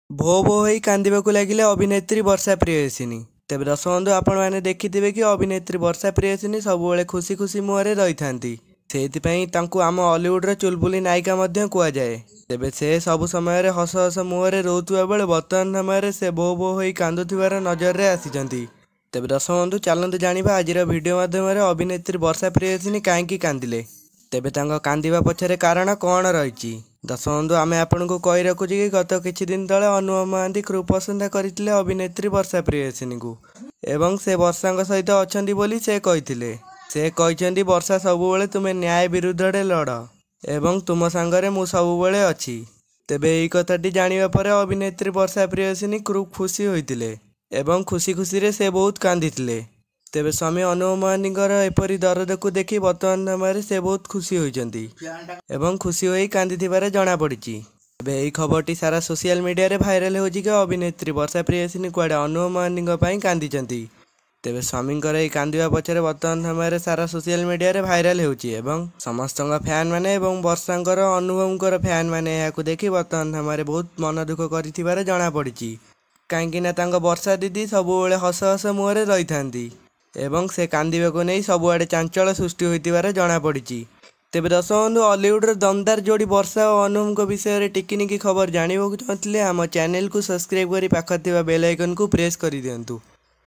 ଅନୁଭବ ଙ୍କ କଥା ଶୁଣି କାନ୍ଦିଲେ ବର୍ଷା ll Varsha Priyadarshini was crying for Anubhav mohanty ll
ଅନୁଭବ ଙ୍କ କଥା ଶୁଣି କାନ୍ଦିଲେ ବର୍ଷା ll Varsha Priyadarshini was crying for Anubhav mohanty ll Creative news